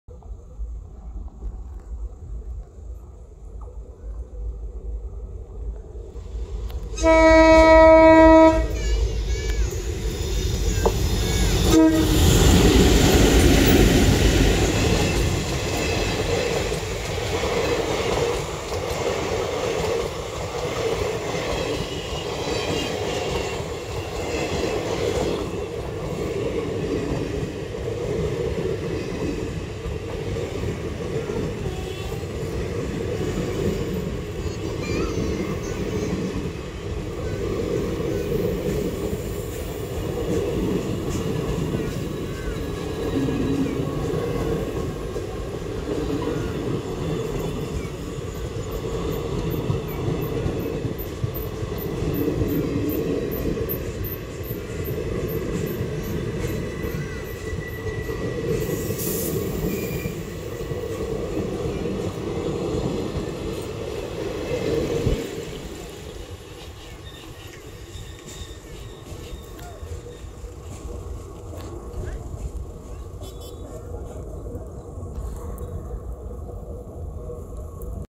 17620 Hazur Sahib Nanded - Aurangabad Weekly Express | Twin WDM3A Locomotive | Indian Railway Video
17620 Hazur Sahib Nanded - Aurangabad Weekly Express

Locomotive - WDM3A
Coach - Utkrisht Coaches

Train 17620 NED AWB EXP runs from Huzur Sahib Nanded(NED) to Aurangabad(AWB) on Friday. Train departs from Huzur Sahib Nanded(NED) at 11:50 and reaches Aurangabad(AWB) at 16:50. It takes 1 day to reach train 17620 from Huzur Sahib Nanded(NED) to Aurangabad(AWB) .

NED AWB EXP 17620 train passes through 8 major stations in 1 day to reach from Huzur Sahib Nanded to Aurangabad . It takes total of 5:0 hours journey to reach Aurangabad .

In 17620 train route maximum halt time for this train is 5 minutes at Purna Jn(PAU). Train 17620 arrives at Purna Jn at 12:15 and departs at 12:20. So if you want to get down from the train and buy anything from the station or for little fresh air. It's safe to get down at this station.